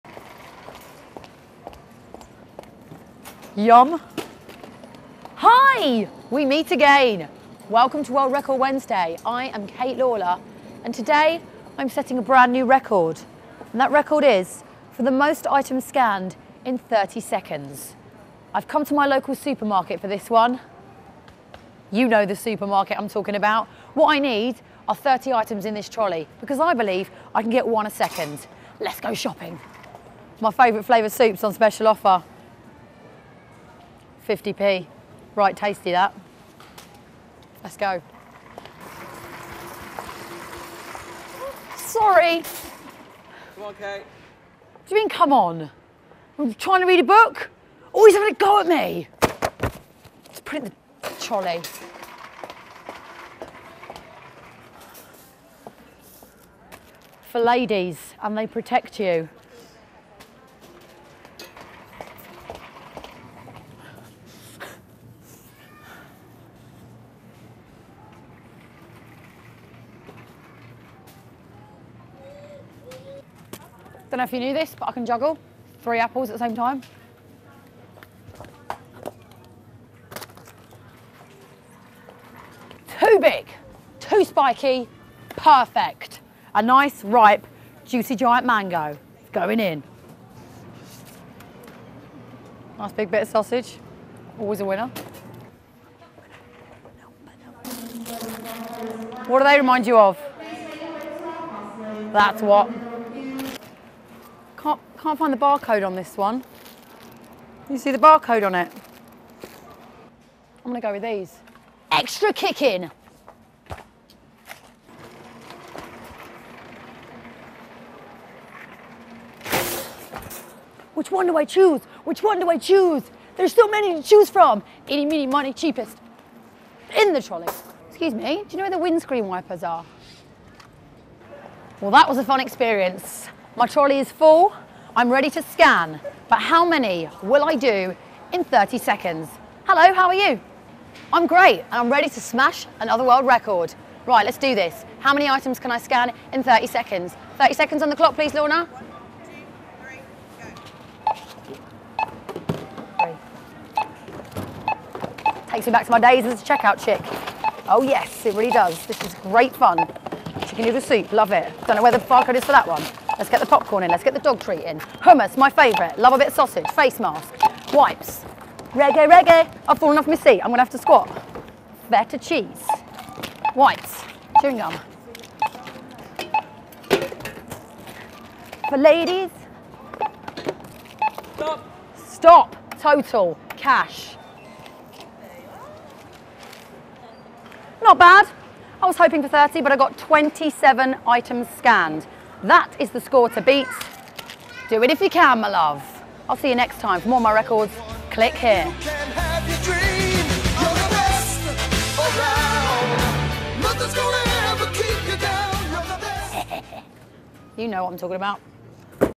Kate Lawler | Most Items Scanned at Check-out in 30 seconds | World Record Wednesday
Kate sets the record for the most items scanned at a supermarket check-out in 30 seconds.

Beat it if you can!